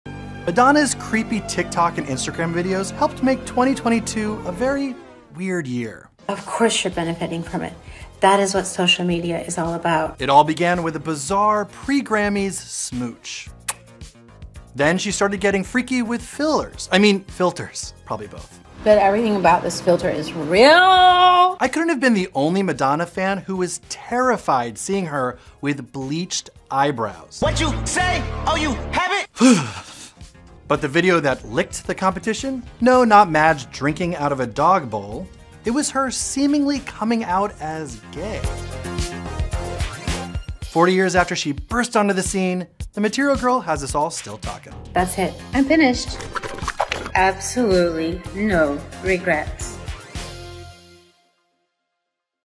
Madonna’s most bizarre TikTok and Instagram videos of 2022 | Page Six Celebrity News
From all the freaky filters, to drinking out of a dog bowl, we are taking a look back at all of Madonna’s creepy TikTok and Instagram videos that helped make a bizarre 2022!